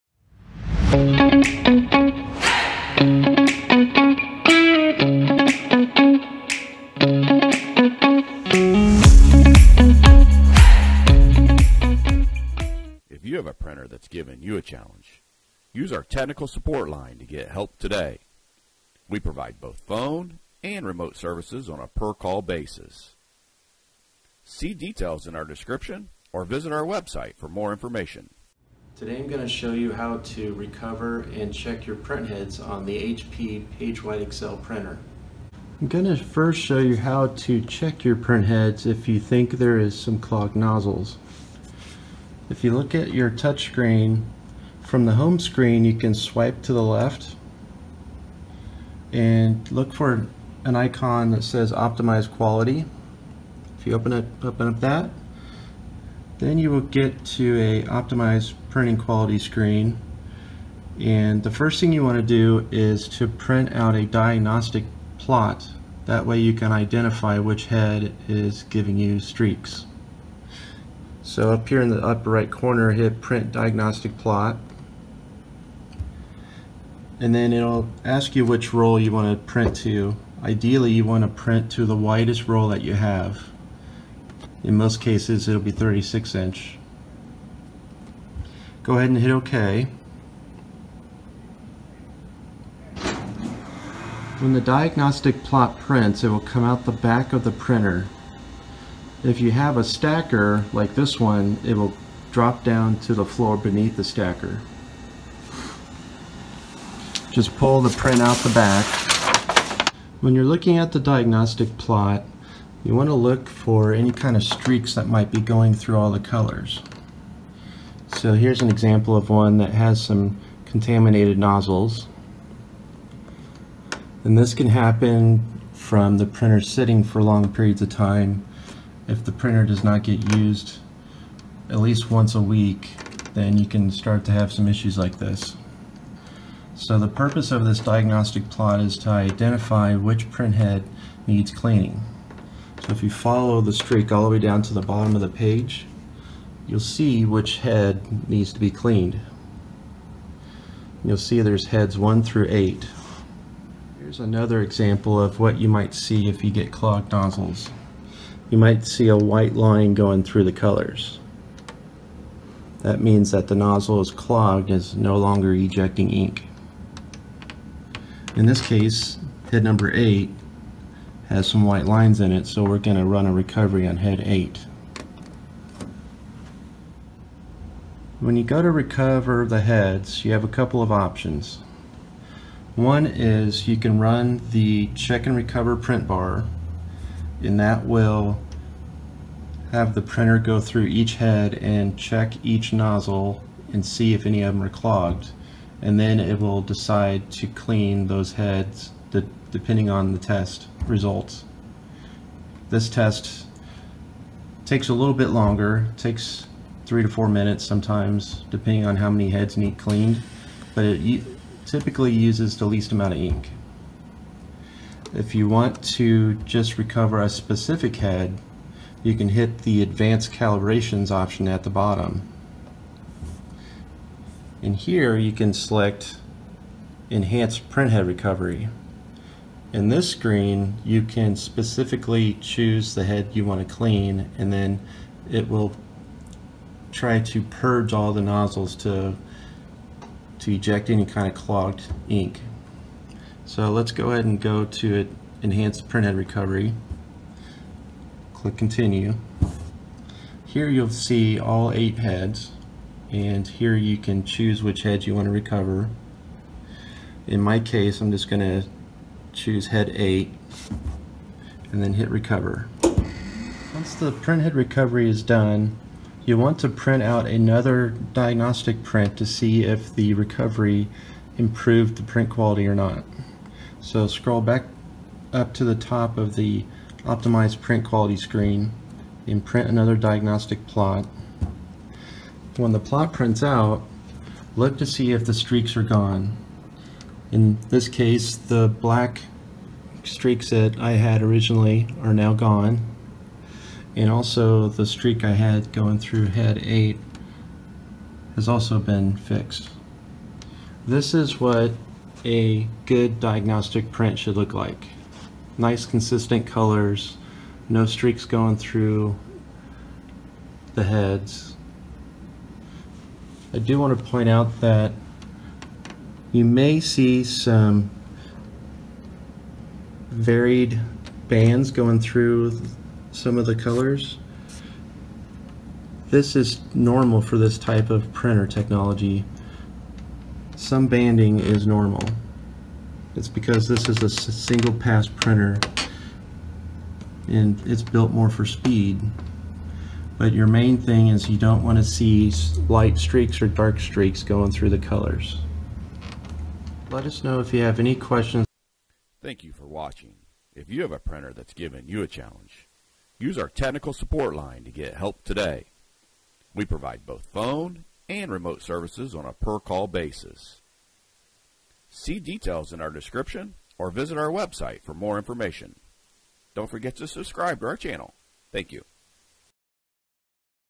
HP Pagewide XL - How to Calibrate Printhead
Learn how to calibrate a HP PWXL printhead properly. Learn the mistakes and what to look for to do this properly. We take you step by step in a live demonstration. Please visit our service homepage and contact form for help with you device today